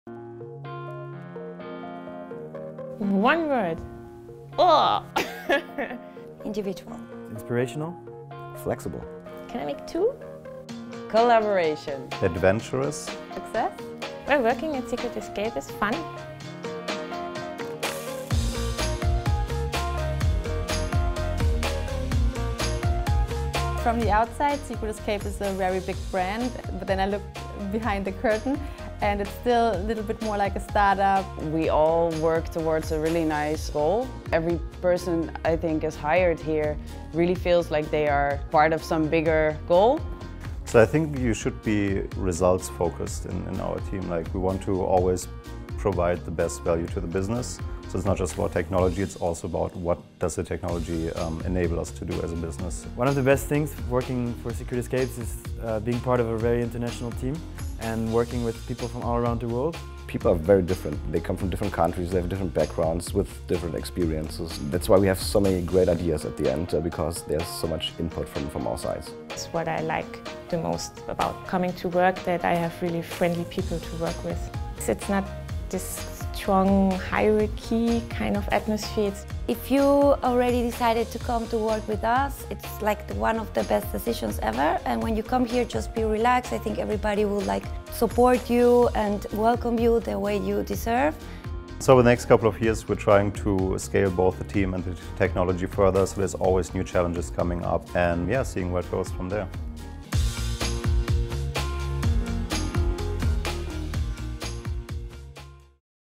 Life at Secret Escapes Berlin | Jobbio
The team at Secret Escapes Berlin described the company as inspirational, adventurous, and most importantly, fun!

Make one of your best decisions and apply for one of their open roles